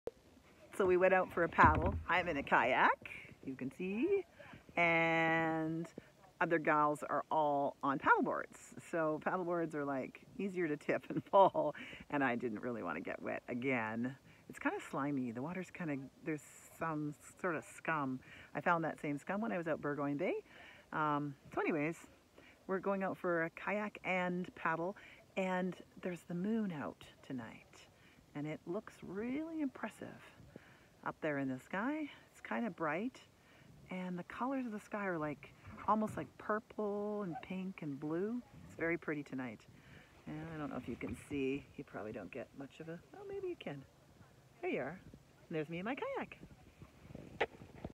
Kayaking and Paddle boarding in Long Harbour
A kayak and paddle board in long harbour BC Salt Spring Island